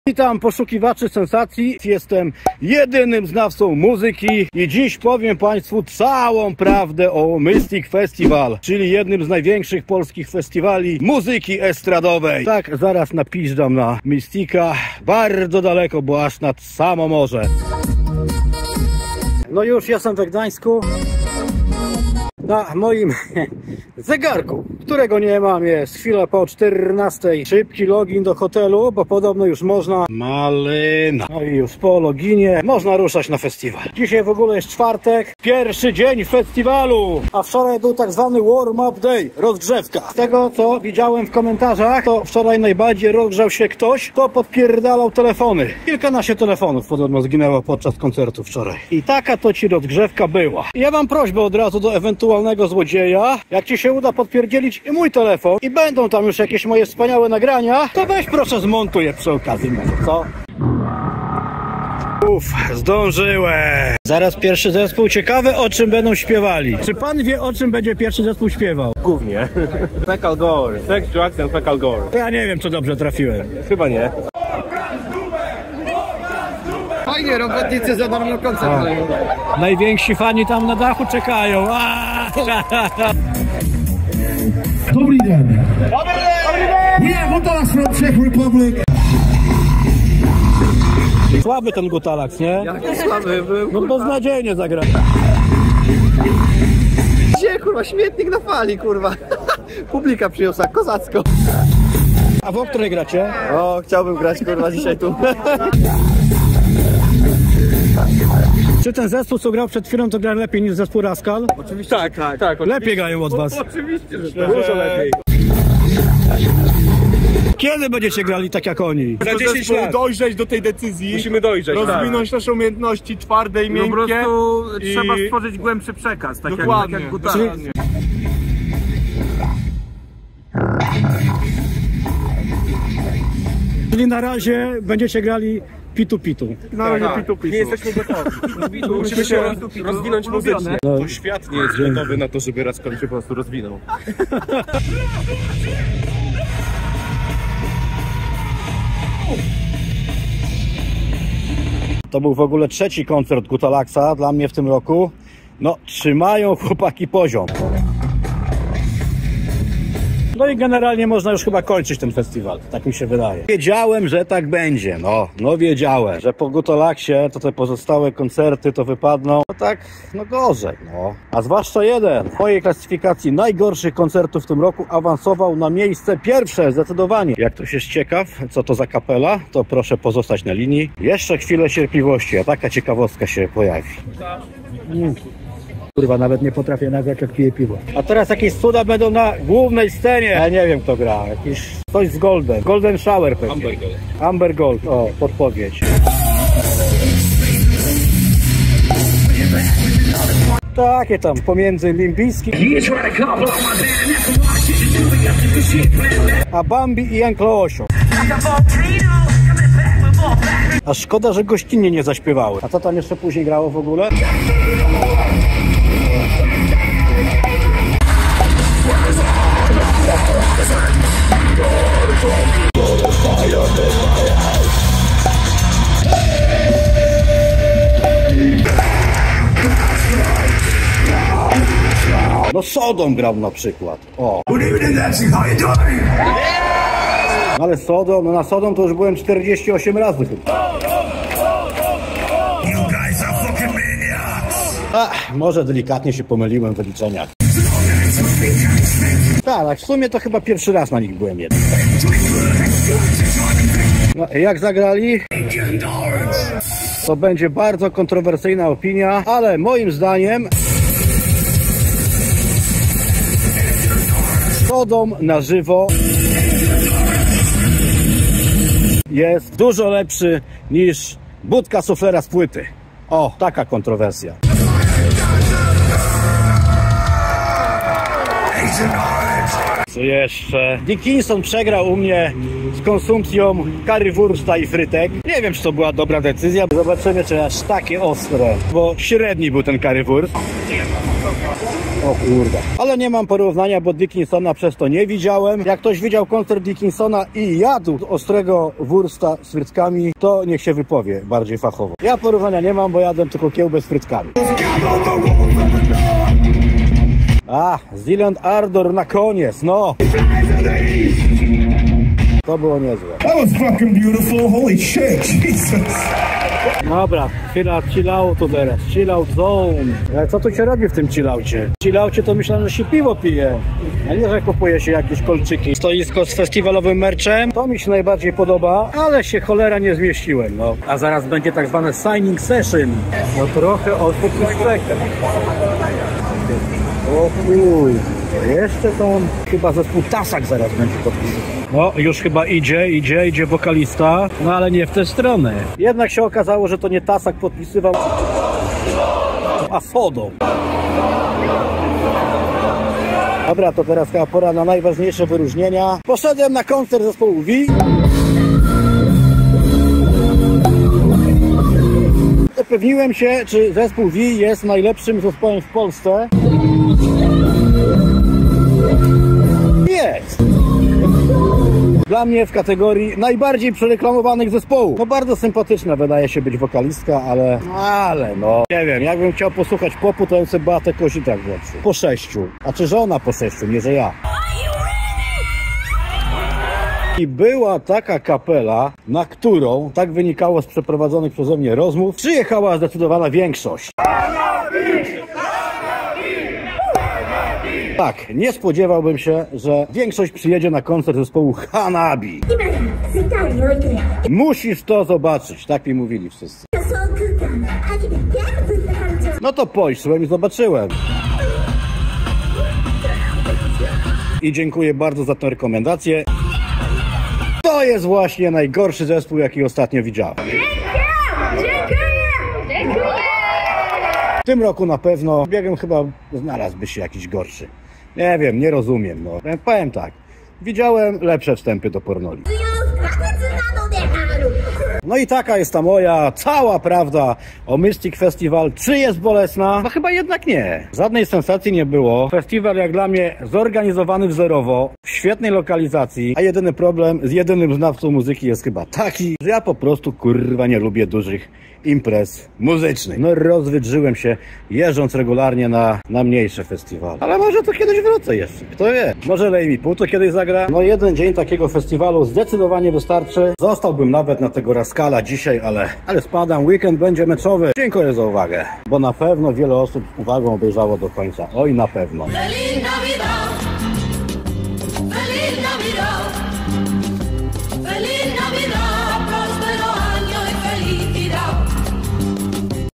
🟢 Cała Prawda o MYSTIC FESTIVAL 2024 🟢
Reportaż publicystyczno-naukowy z tegorocznej edycji MYSTIC FESTIVAL.